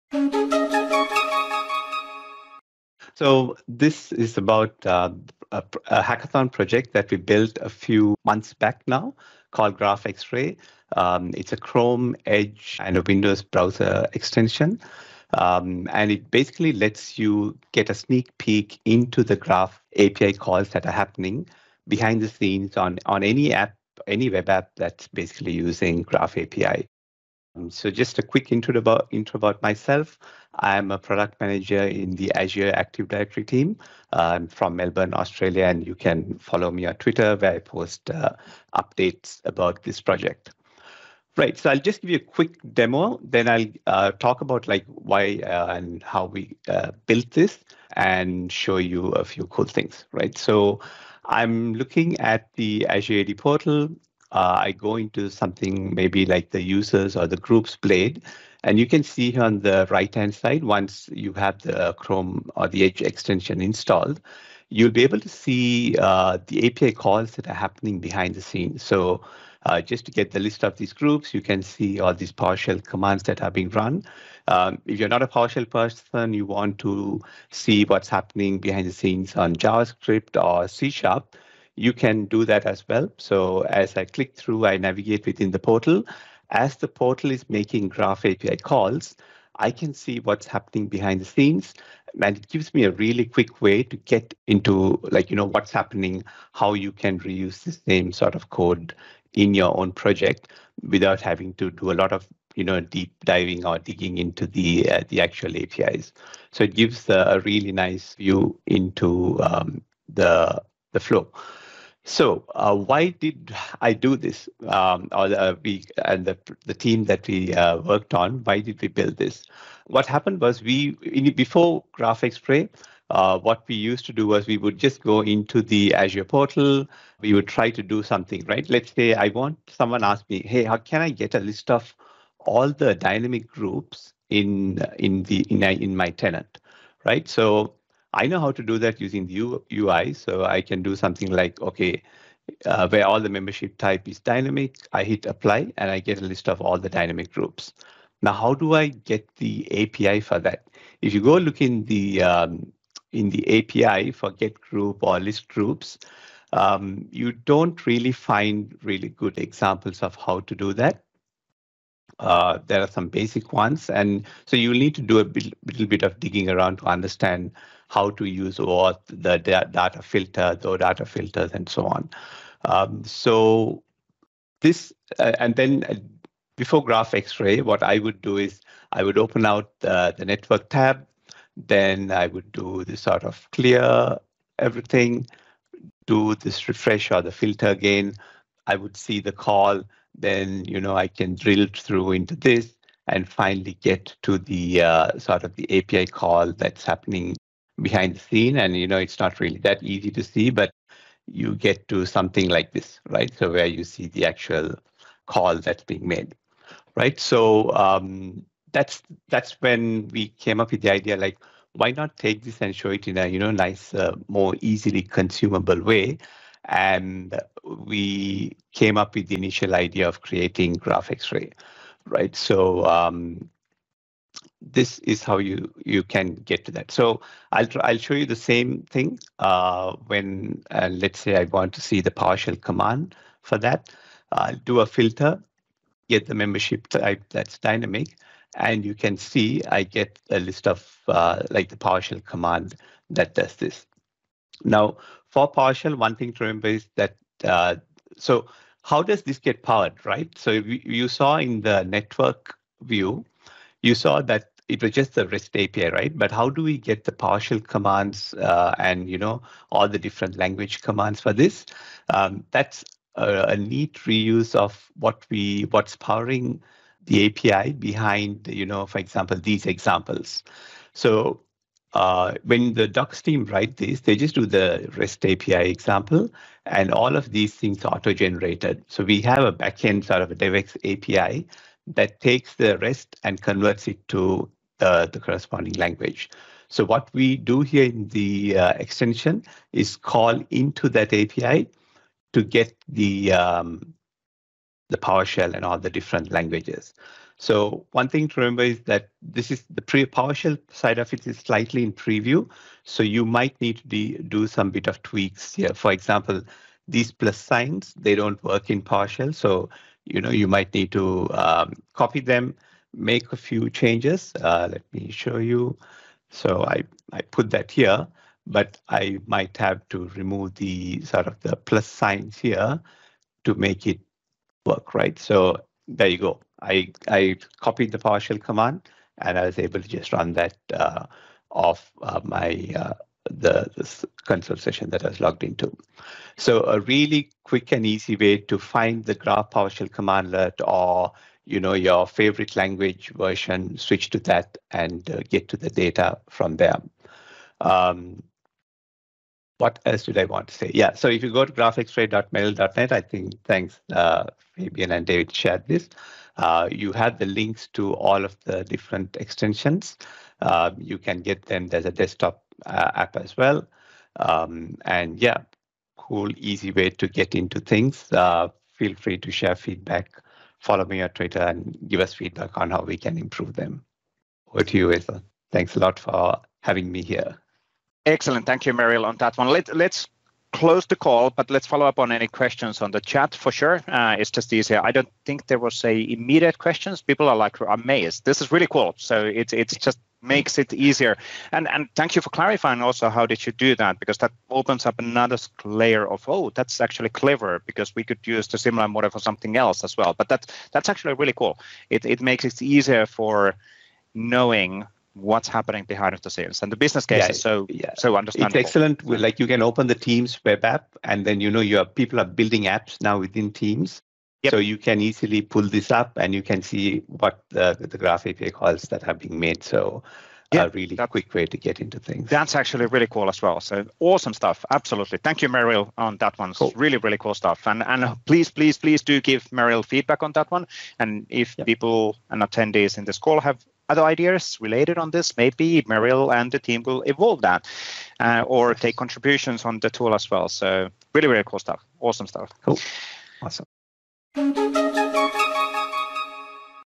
Introduction to Microsoft Graph X-Ray project
In this 7-minute developer focused demo, Merill Fernando and team delivers Chrome, Edge and Windows Browser extensions for going from Microsoft Azure Active Directory Portal actions to Microsoft Graph PowerShell in seconds. Step through use case - how get a list of all dynamic groups inside my tenant.  Address case the old way and then using Microsoft Graph X-Ray that shows used PowerShell, JS, Java, C#, Objective-C, and Go commands. Allows you to understand and reuse code in your project without having to deep dive into the APIs. This PnP Community demo is taken from the weekly Microsoft 365 Platform Community call recorded on September 6, 2022.

Demo Presenter
•  Merill Fernando (Microsoft) | @merill